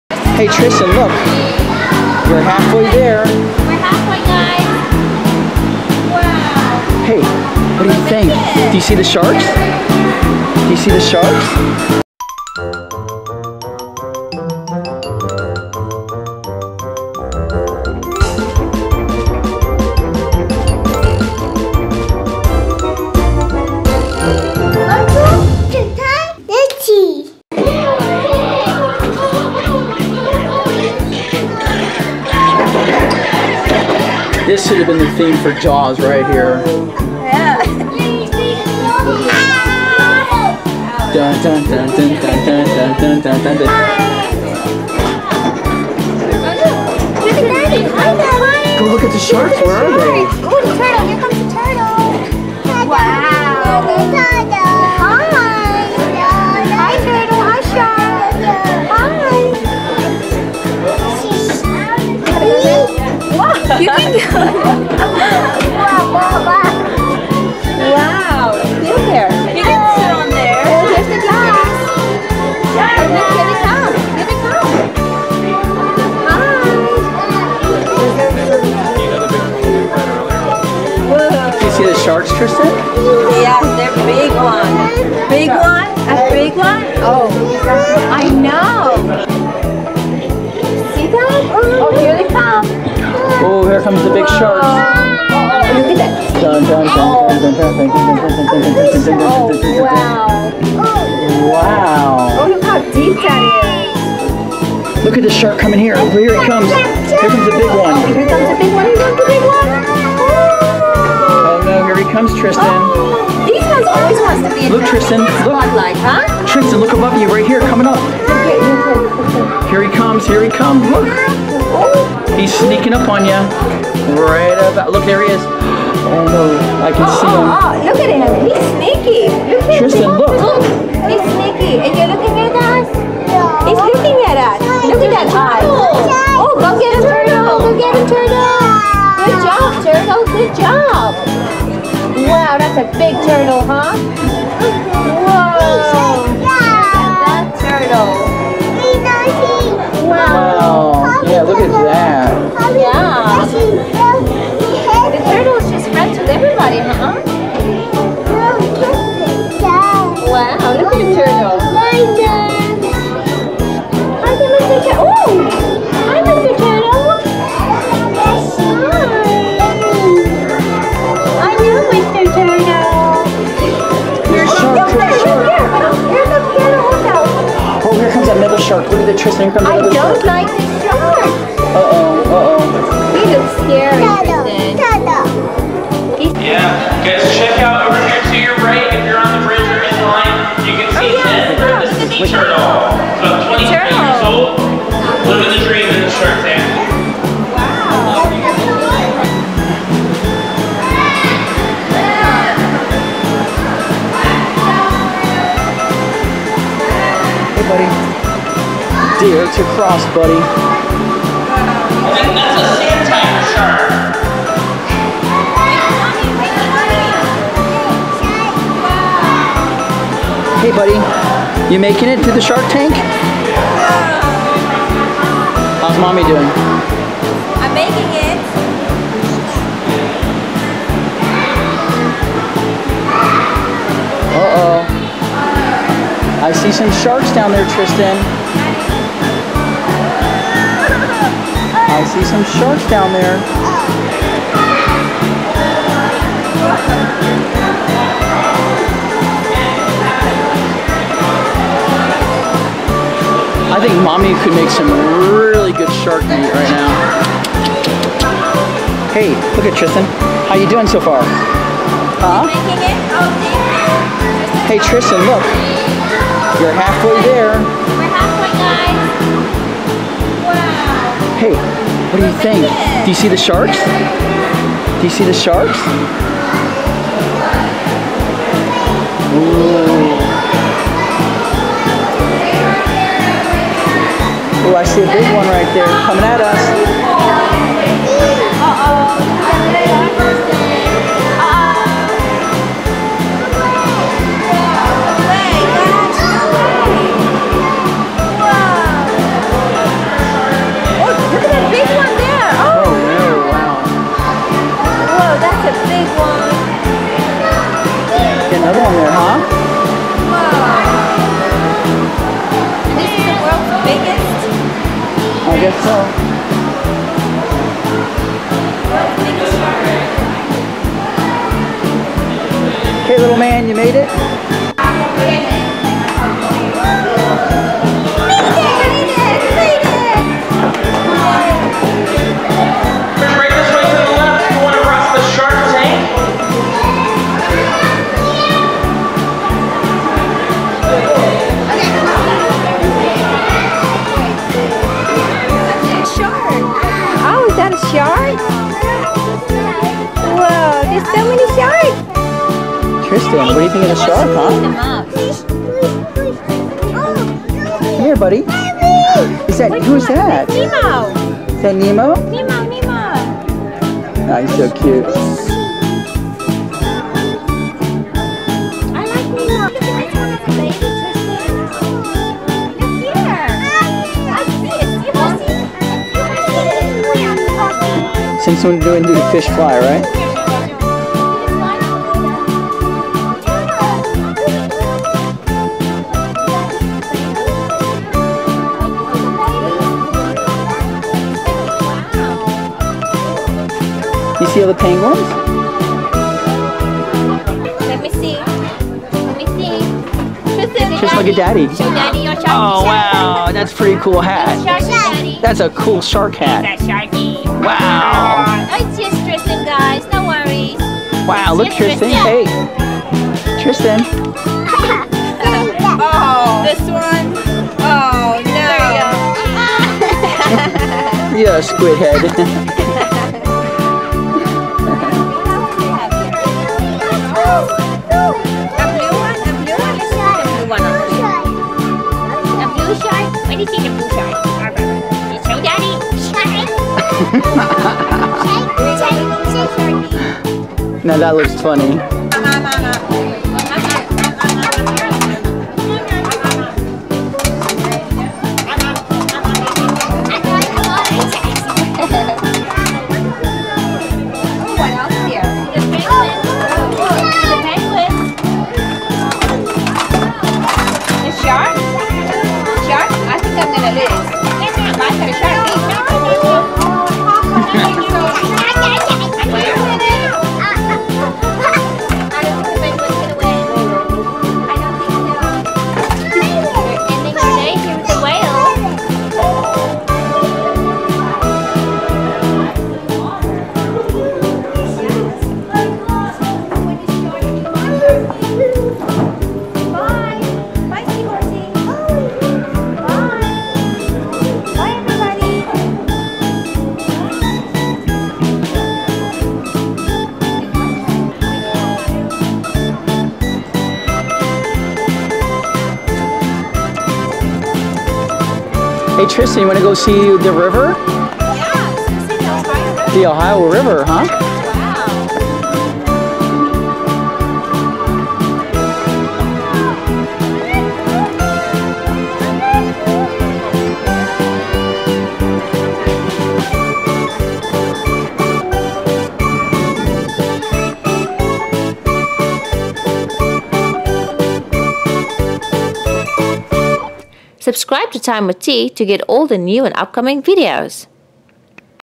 SHARK BRIDGE CROSSING! and more SHARKS! PART 3 OF 3
AQUARIUM FUN Shark bridge sea turtles kids explore PART 3 OF 3 in this family fun adventure where Tristan and his family cross over a 75 foot shark bridge just inches above nearly 2 dozen sharks. What a great and amazing experience sharing it with everyone!

To watch the previous PARTS of this EPISODE "AQUARIUM FUN" see below and click on the links.